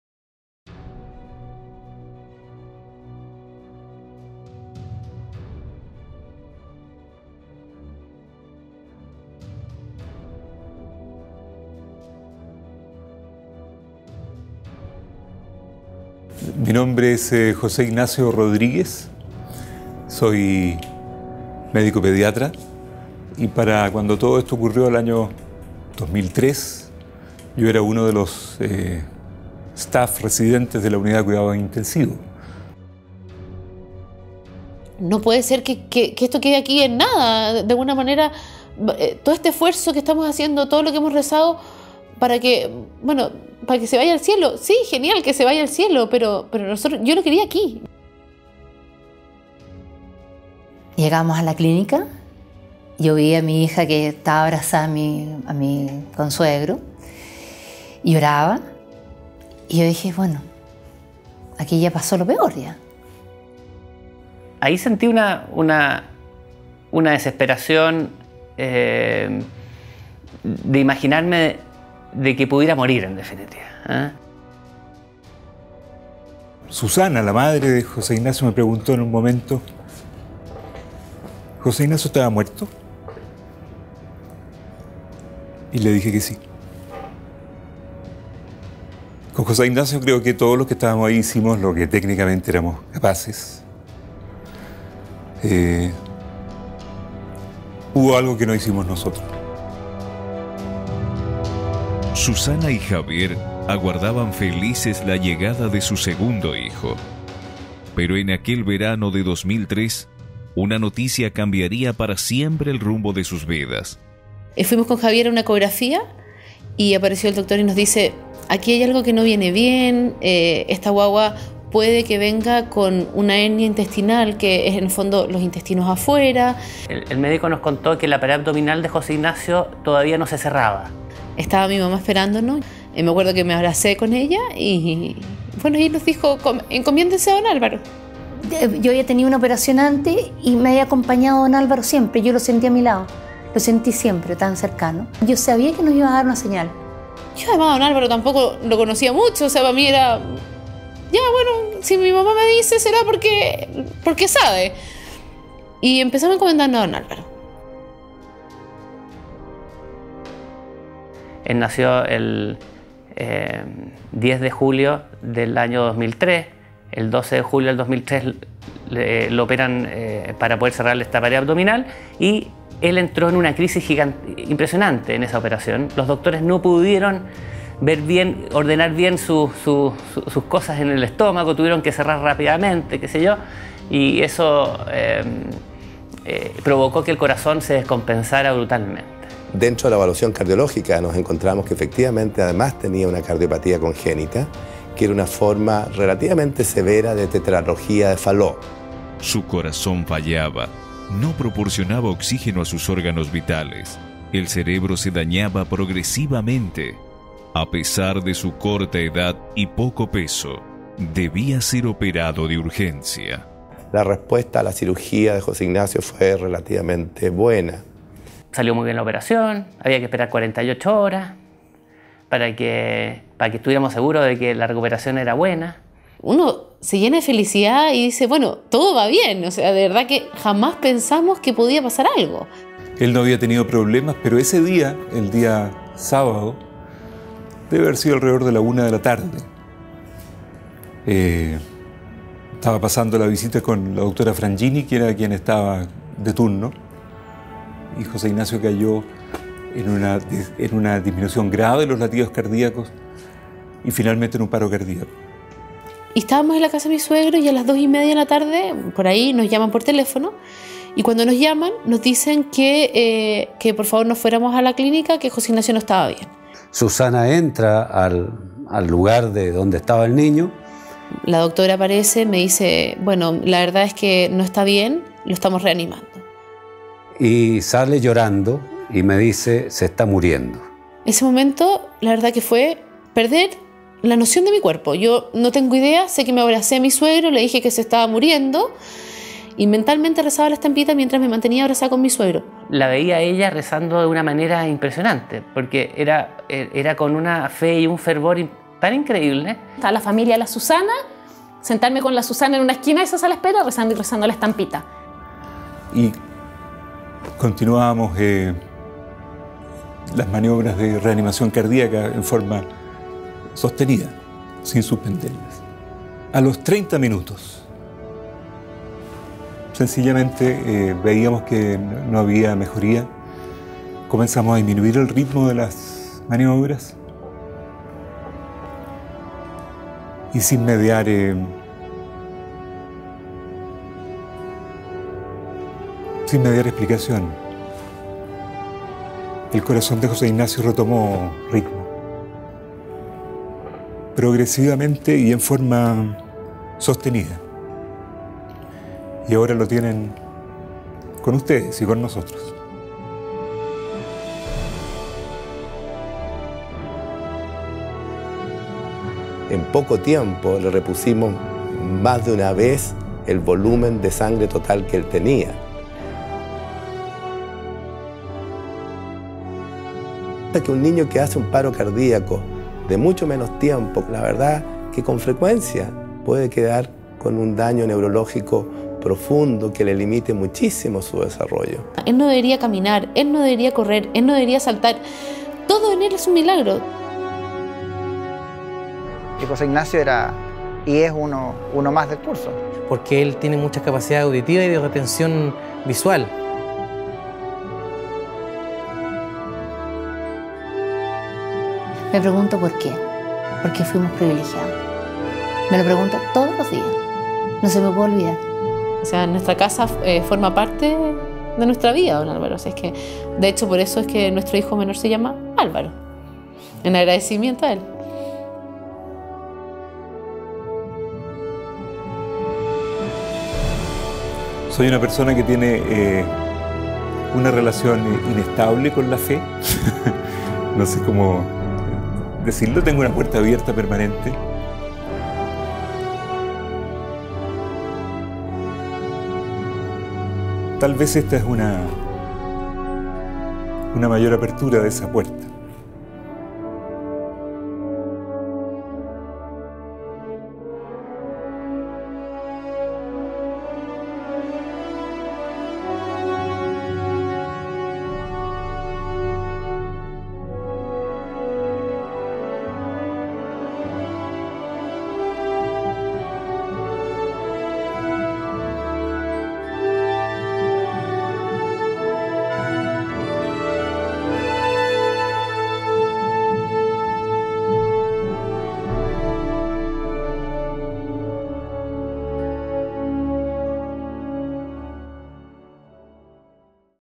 Don Alvaro's miracle (English version)
This is the amazing and moving story of the coming back to life of an infant, Jose Ignacio Ureta Wilson, who suffered a massive hemorrhage and whose heartbeat stopped for 30 minutes. He came back to life through the intercession of Bishop Alvaro del Portillo. But Bishop Alvaro didn't only procure the boy's life back - he did more. This video shows you the details of that story.